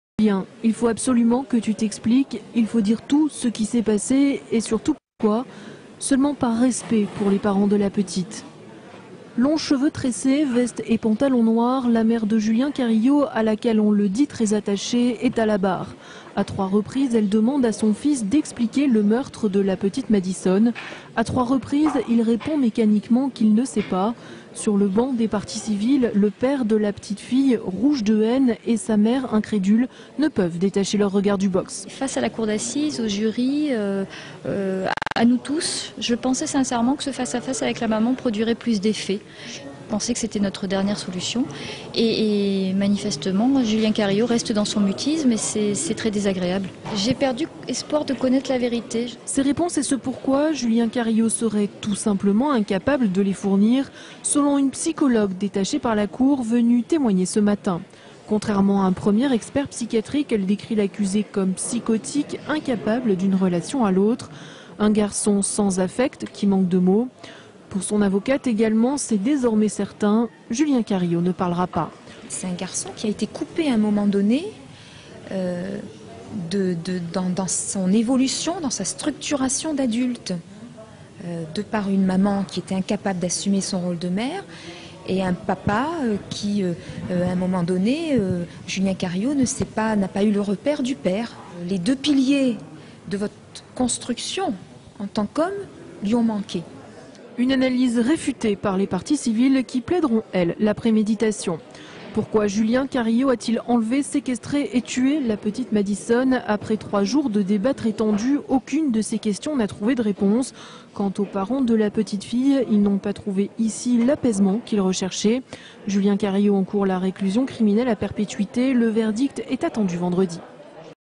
LCM Marseille - Affaire Madison : une journée éprouvante
30/04/2008 - LCM Marseille - Affaire Madison : une journée éprouvante - Marseille - Troisième jour de procès aux Assises d'Aix-en-Provence dans l'affaire du meurtre de la petite Madison. C'est la mère de l'assassin présumé, Julien Carillo, qui est venu témoigner aujourd'hui sans que cela puisse réellement faire avancer les débats. Il ne reste plus qu'une journée au prévenu pour expliquer ses actes. (Pour obtenir les droits d’exploitation commerciale de cette vidéo, veuillez contacter )